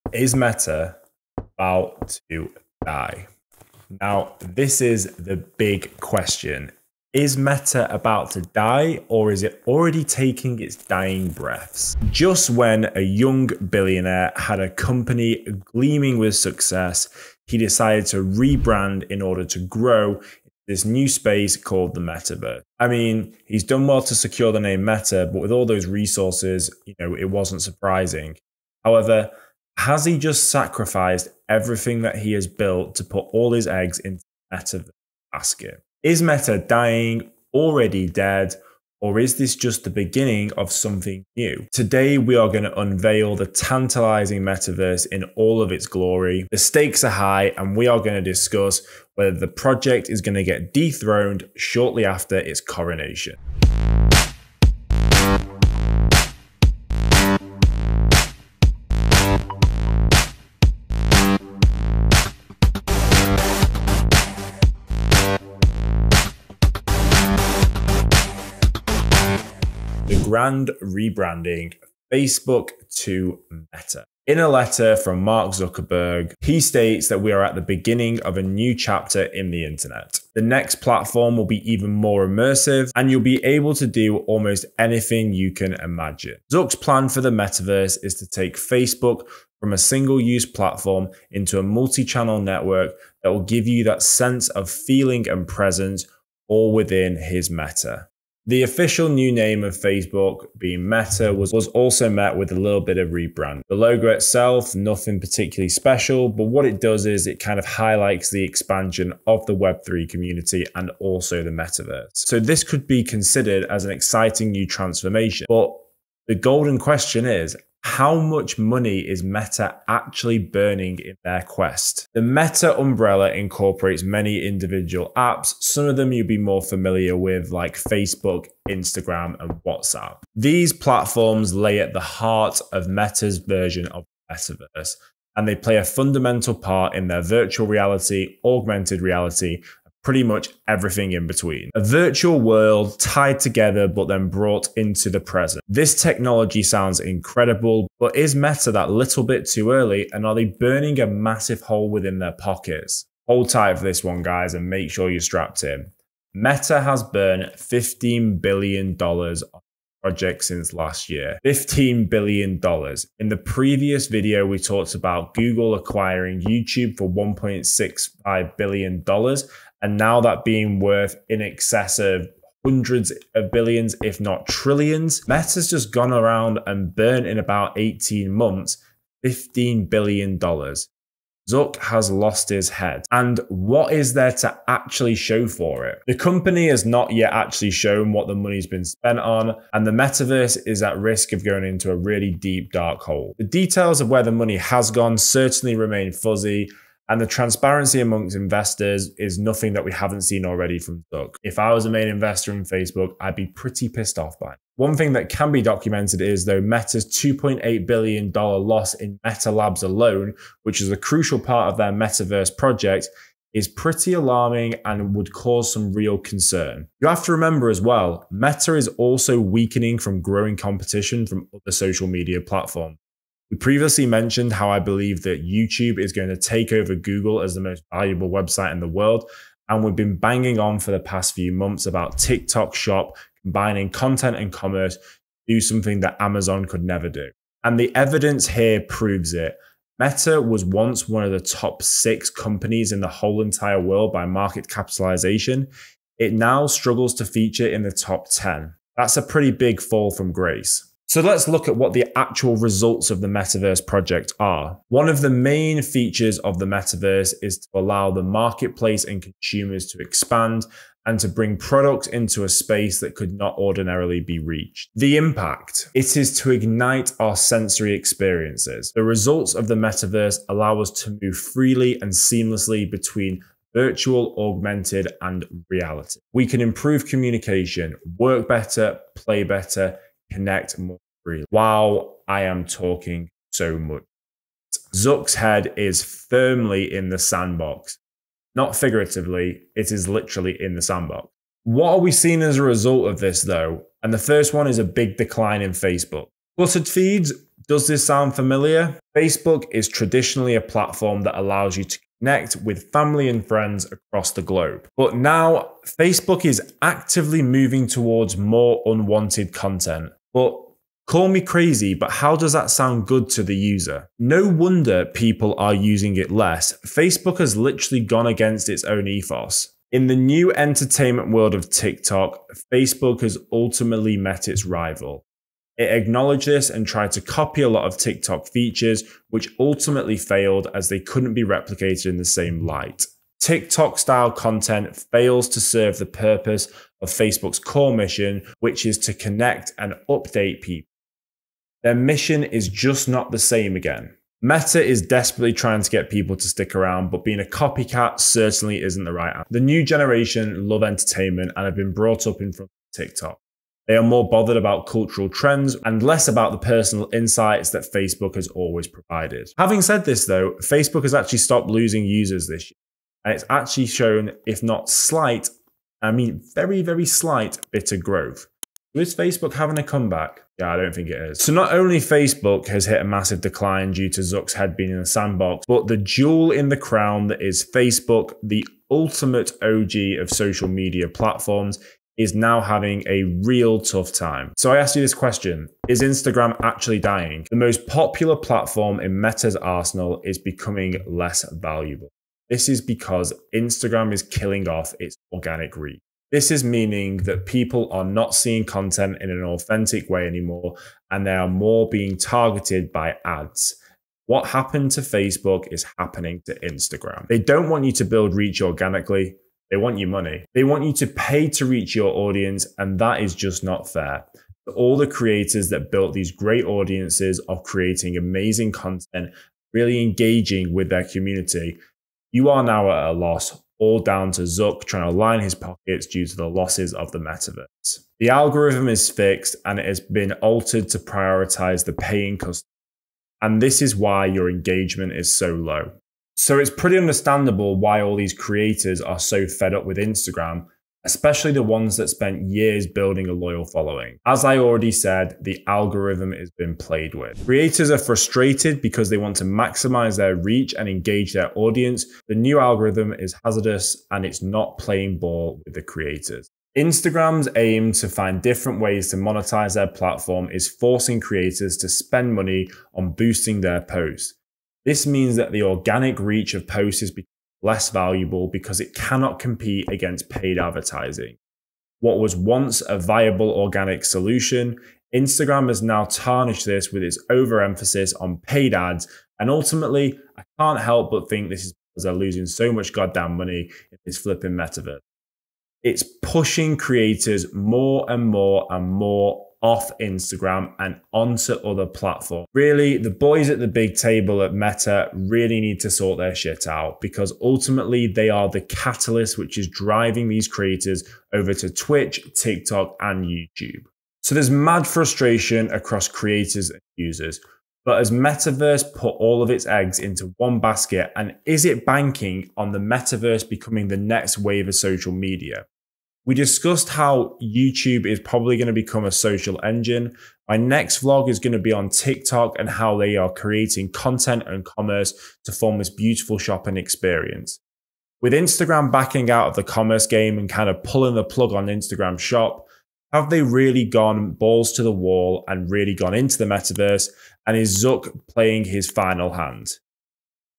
Is Meta On The Verge Of Collapse
Meta and Mark Zuckerberg have been subject to more scrutiny recently with the latest earning reports and Meta's cash burn on their Metaverse project.

Zuckerberg's quest to built the next virtual reality is significantly damaging Meta, with its market cap being absolutely pounded. Whilst Meta's attention has firmly been on virtual reality and the Metaverse, they have let other social media platforms like TikTok and YouTube completely dominate the space.

Is this the end for Meta, will Instagram and Facebook slowly die out, only time will tell.